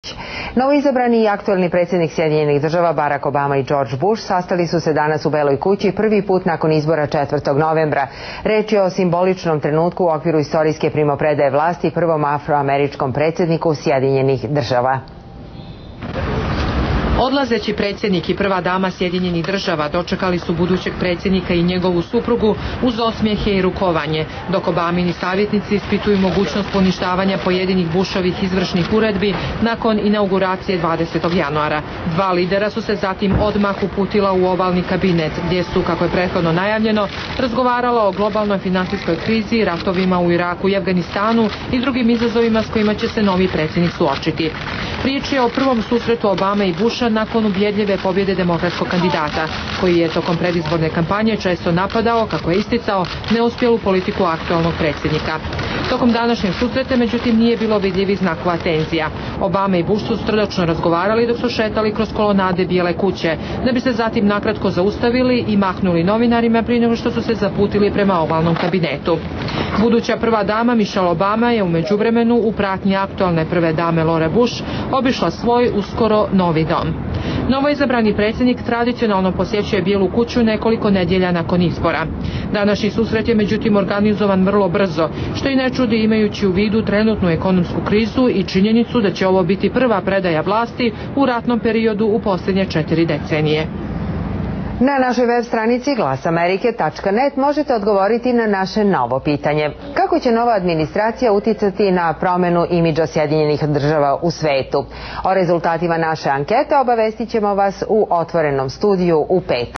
Predsednik Buš i Barak Obama razgovarali u Beloj kući
Novoizabrani i aktuelni predsjednik Sjedinjenih Država Barak Obama i Džordž Buš sastali su se danas u Bijeloj Kući, prvi put nakon izbora 4. novembra.